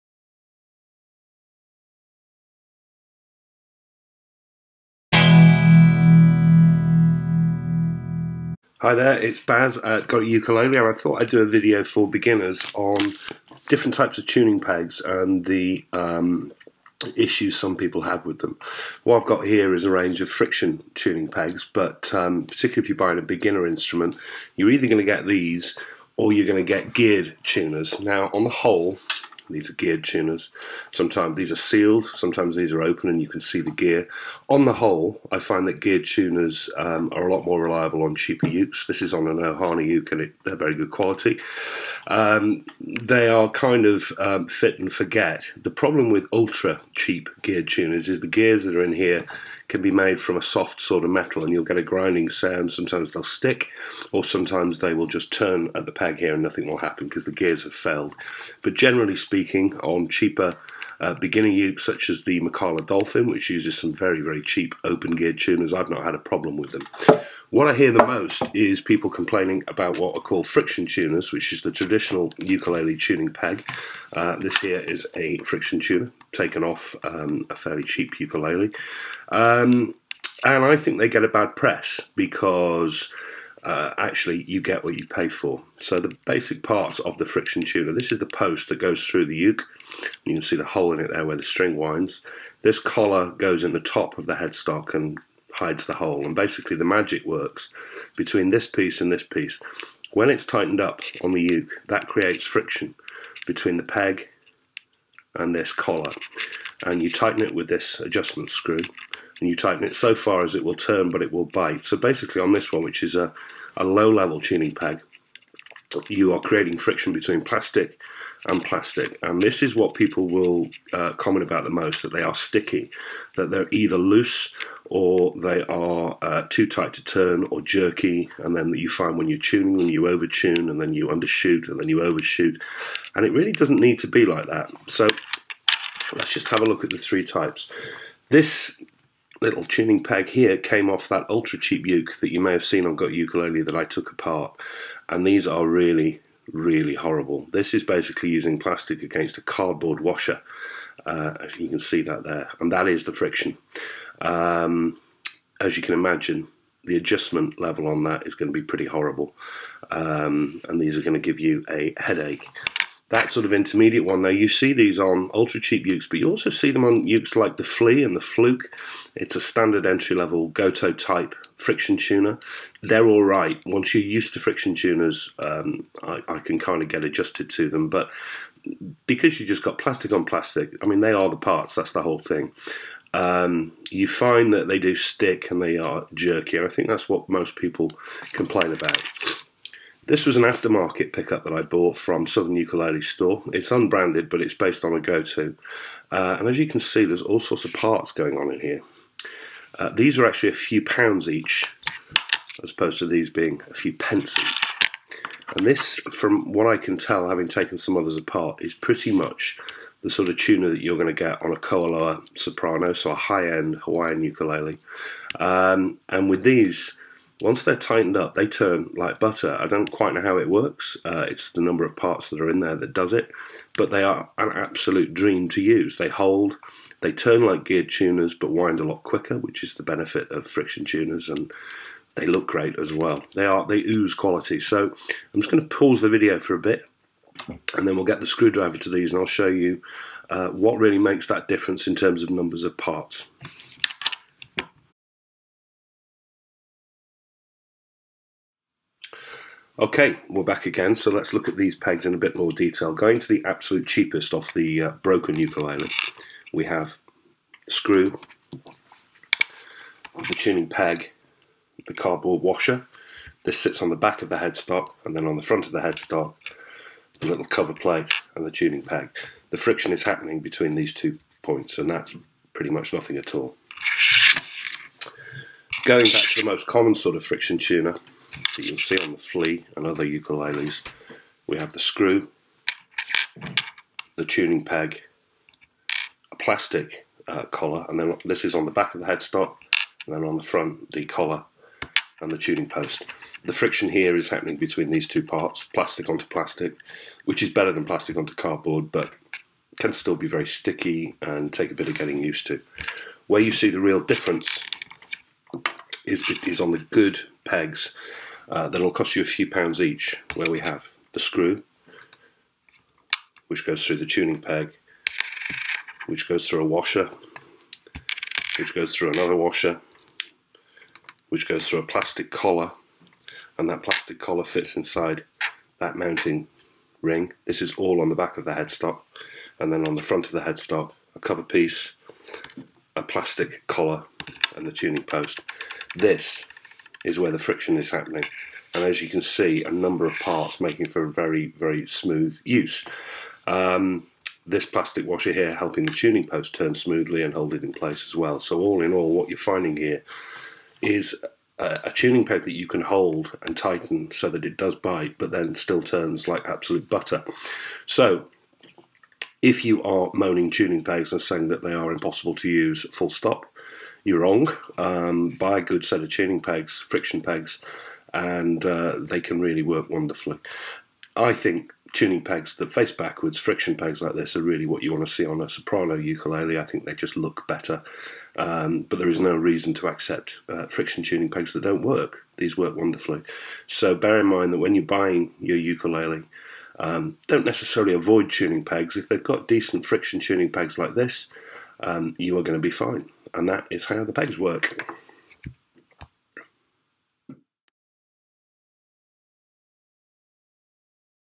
All About Ukulele Friction Tuners - Got A Ukulele Beginners Tips
A beginners look inside friction tuning pegs for ukulele, and a word on why they are not all to be avoided.